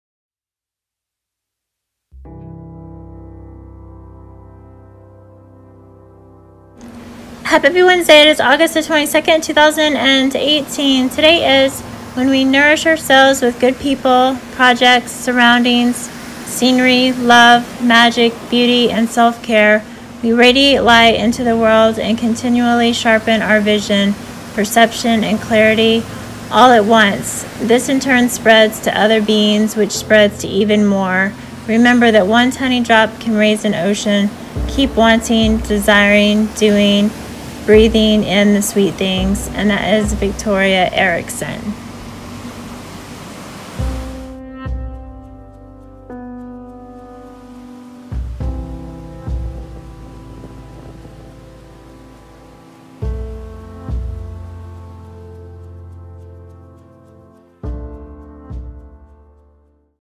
When we nourish ourselves with good people, projects, surroundings, scenery, love... V. Erickson
When we nourish ourselves with good people, projects, surroundings, scenery,  love, magic, beauty and self-care, we radiate light into the world and continually sharpen our  vision, perception , and clarity all at once.  This in turn spreads to other beings which spreads to even more. Remember that one tiny drop can raise an ocean. Keep wanting.  Desiring. Doing. Breathing in the sweet things. Victoria Erickson